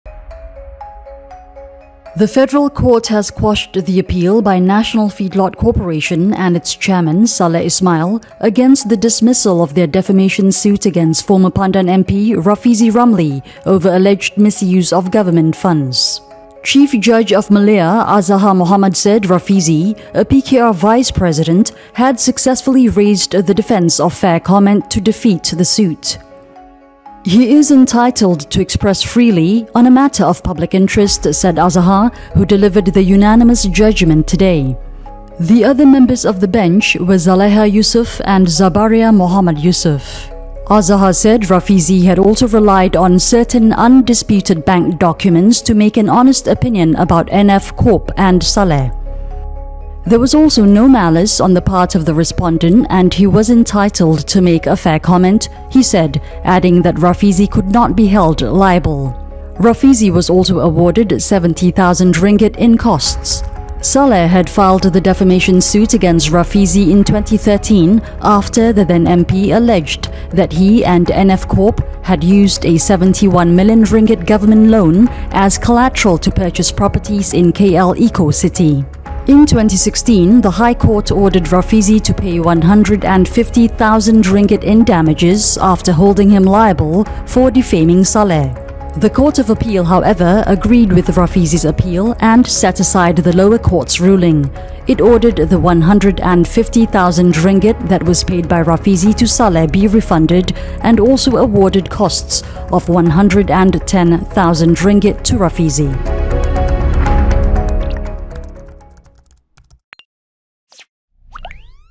NFCorp, chairman lose appeal in defamation suit against Rafizi over alleged misuse of public funds
National Feedlot Corporation and its chairman, Salleh Ismail, lose appeal in defamation suit against former Pandan MP Rafizi Ramli over alleged misuse of government funds.